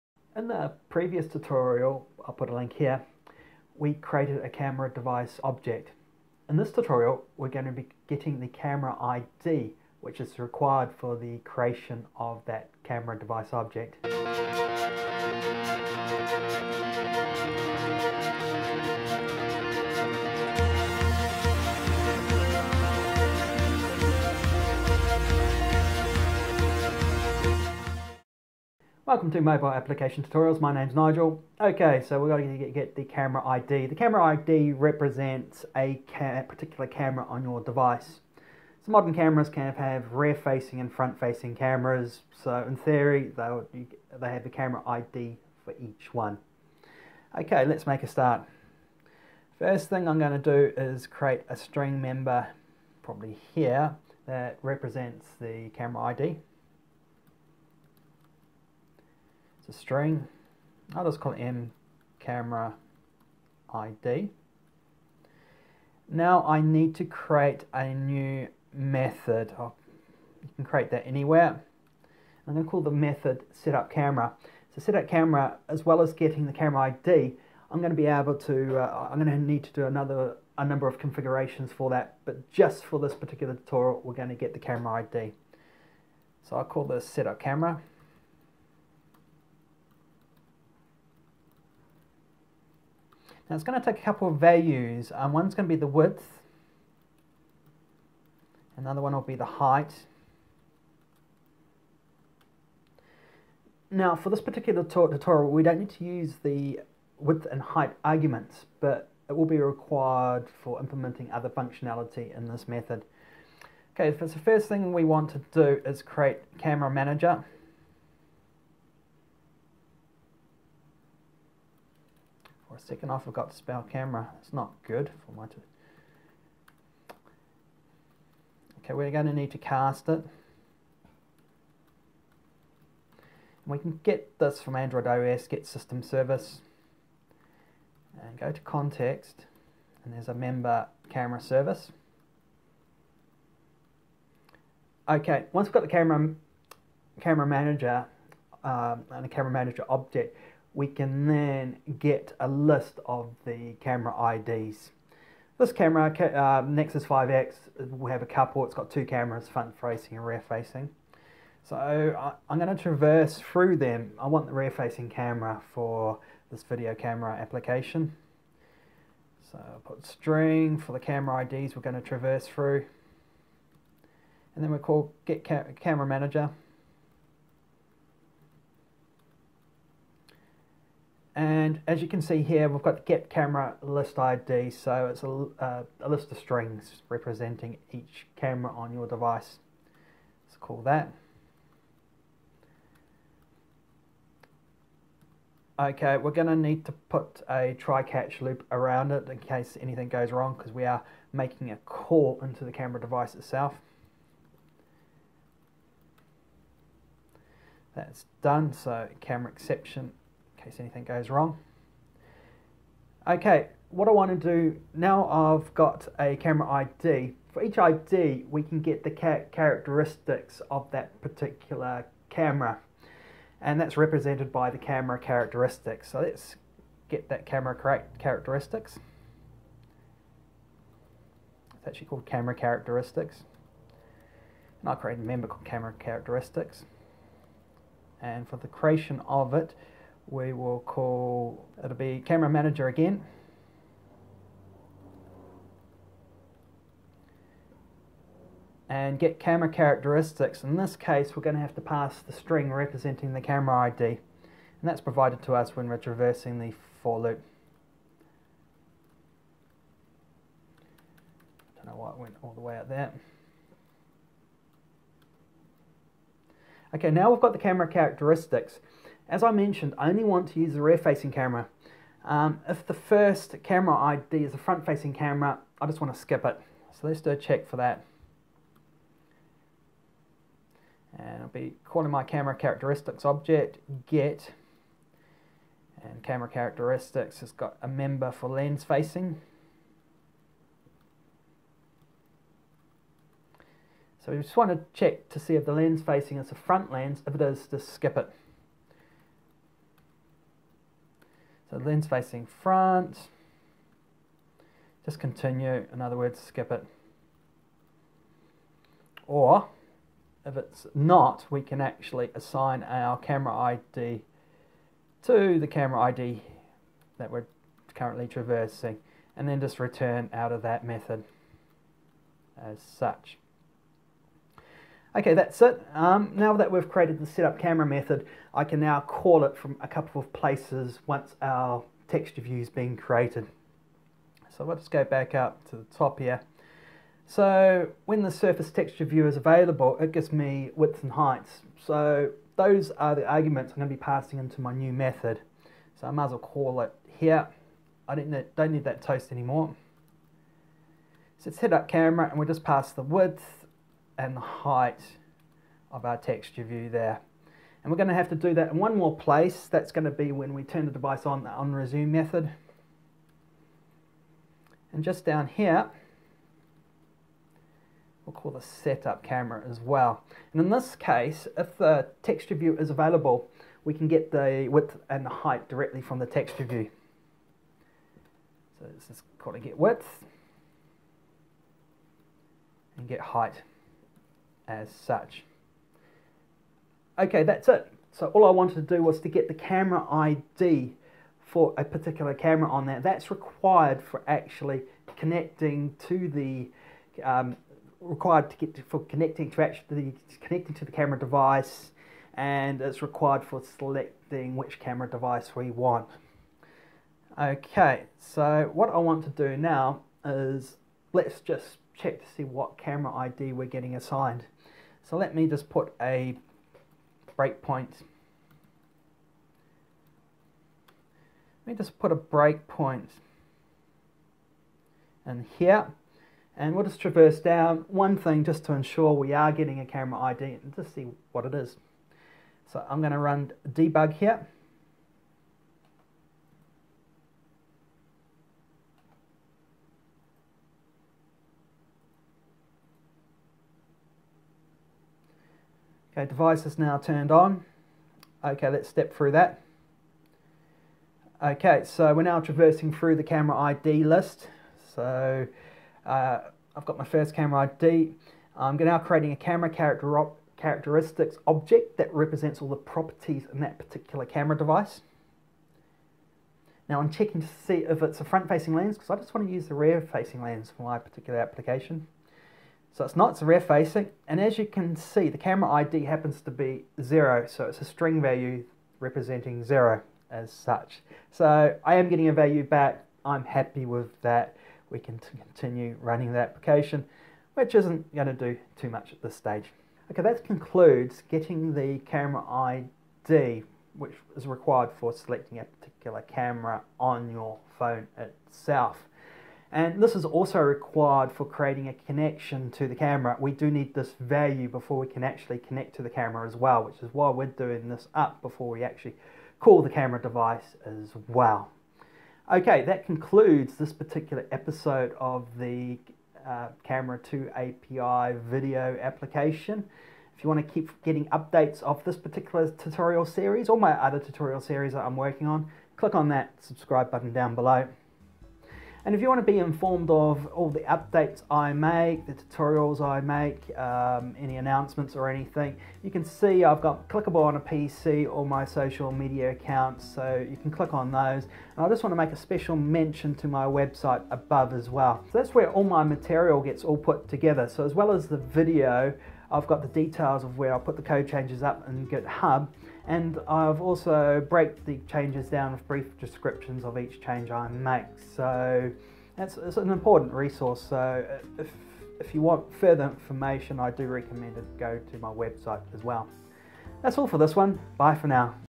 Android Camera2 API Video App - Part 5 Getting Camera Id
Android tutorial series which describes how to create an android video application using the android camera2 API.

Part 5 describes how to get the Id for the rear camera on the device. This camera id is required for making a connection to the rear camera.

Github details:

You can get it from here

or you can run this command

In this tutorial we will cover:
- Calling the CameraManager
- How to get a list of the camera id's
- How to get the CameraCharacteristics object for a particular camera id
- How to identify the front facing camera
- Setting the camera id

API's covered
- CameraManager
- CameraManager.getIdList
- CameraManager.getCameraCharacteristics
- CameraCharacteristics.LENS_FACING
- CameraCharacteristics.LENS_FACING_FRONT

The previous tutorials were aimed at beginners to android but over time have been getting more complex as the original camera_intent app has developed.
This tutorial series is more aimed towards the intermediate developers thought for those who have been following the previous tutorial series, feel free to watch this android performance series.

Android studio (1.5) is the editor of choice (IDE) for this tutorial series.

This tutorial series is the start of what I expect to be many so I encourage comments and feedback to improve the quality and viewing experience.